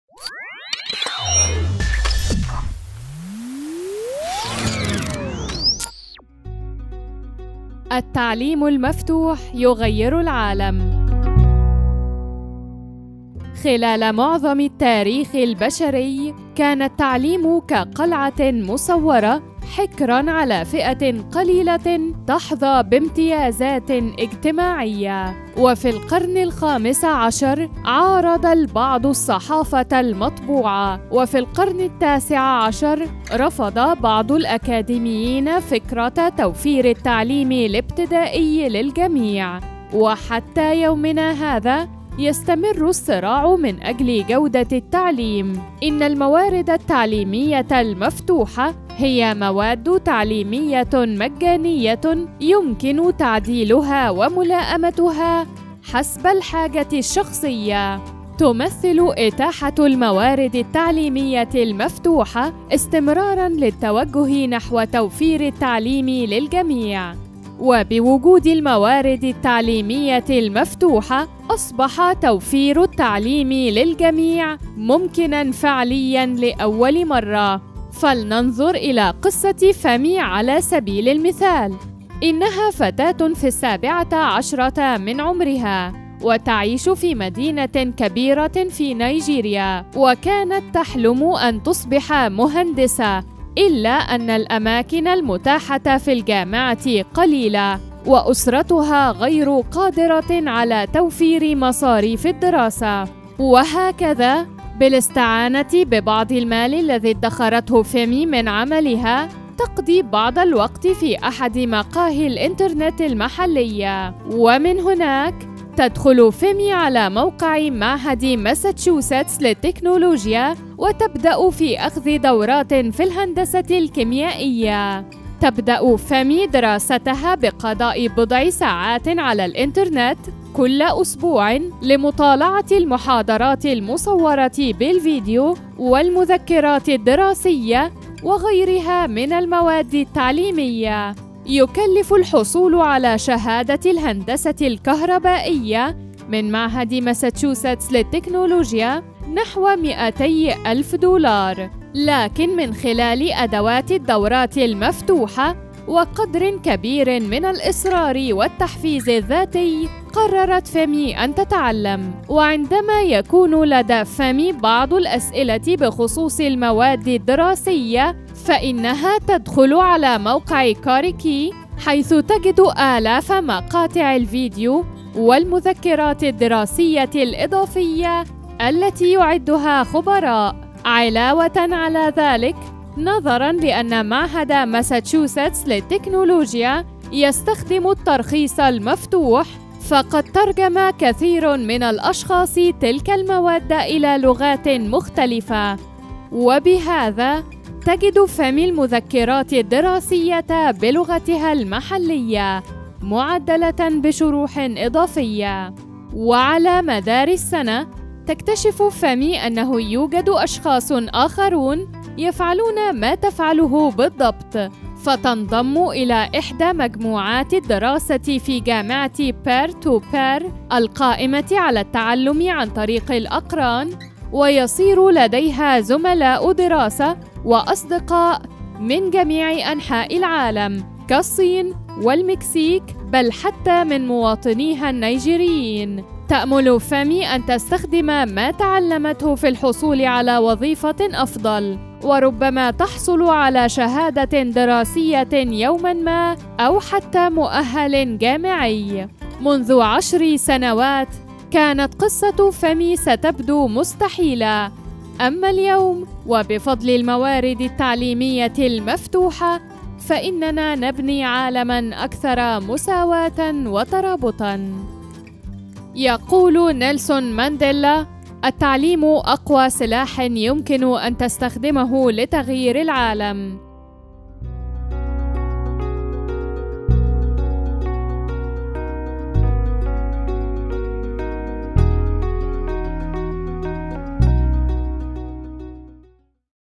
التعليم المفتوح يغير العالم (الجزء الأول)
بفضل الموارد التعليمية المفتوحة، نبني عالمًا أكثر مساواة وترابط ...
شاهد الفيديو وتعرّف على أهمية التعليم المفتوح ...

المصدر: Nadia Mireles